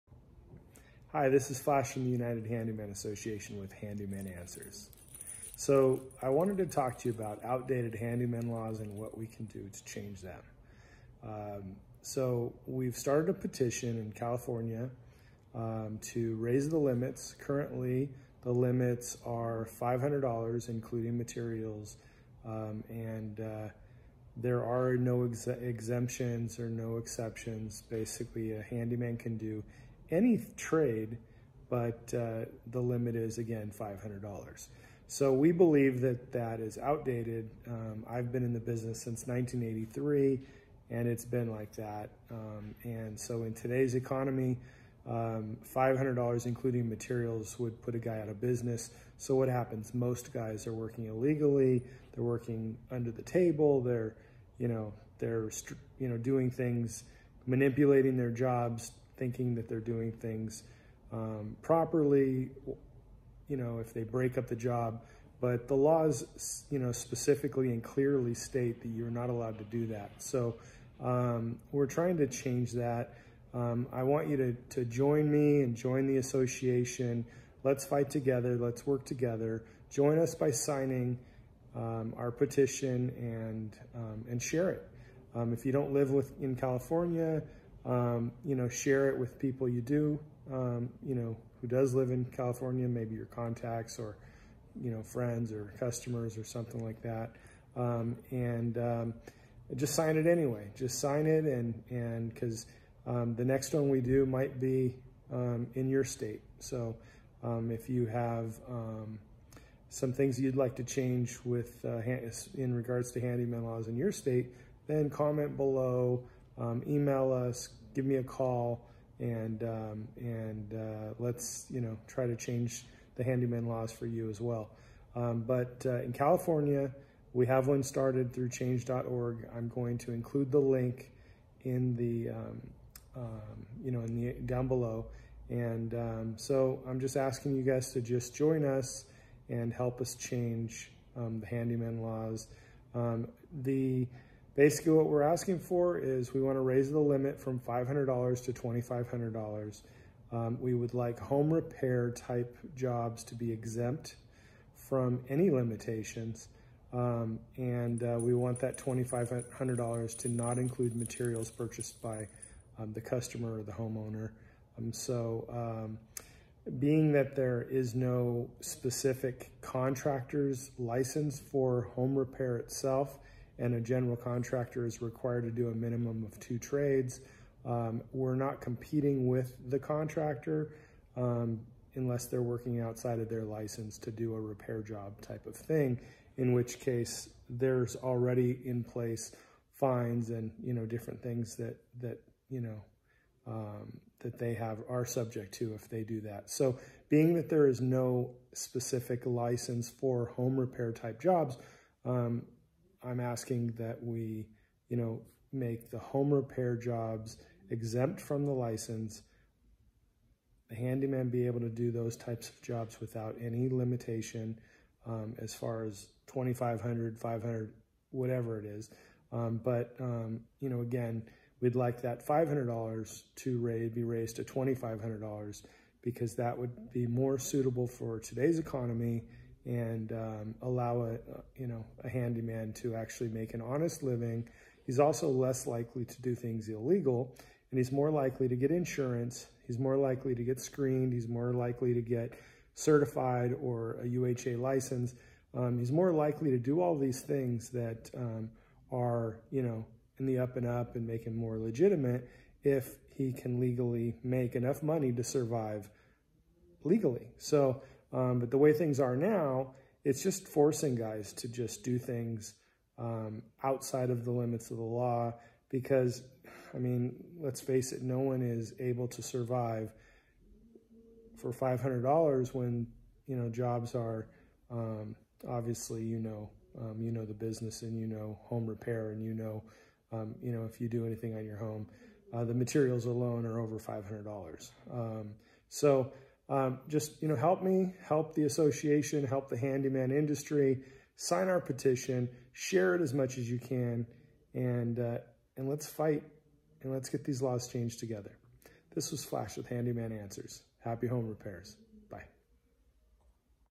Help Change The Handyman Laws
Flash talks about the outdated handyman laws and the petition we started to change them.  Raise limits and make home repair type jobs exempt from license and limitation. Join us in the fight by signing and sharing our petition. The handyman struggles to make a living working within legal limits.